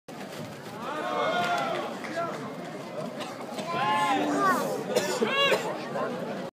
Le Choc des Gladiateurs 2015, Video 6
Kampf der Gladiatoren 2015
Videosequenzen von der Outdoor-Show am Samstag, den 25. Juli 2015 auf dem zentralen Strand Lavandou, Frankreich.